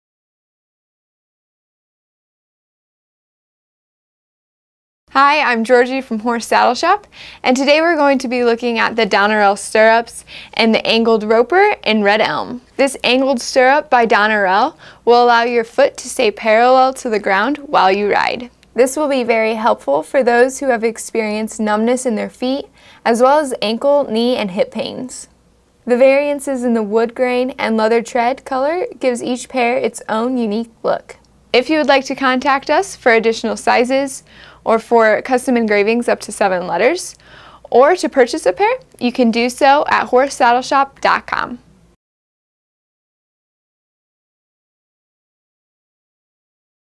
Don Orrell Strirrups Angled Roper Red Elm
This angled stirrup from Don Orrell allow your foot to stay parallel with the ground while you ride. This would be very helpful for those who have experienced numbness in their feet, as well as ankle, knee, and hip pain.

The variances in the wood grain and leather tread color gives each pair it's own unique look.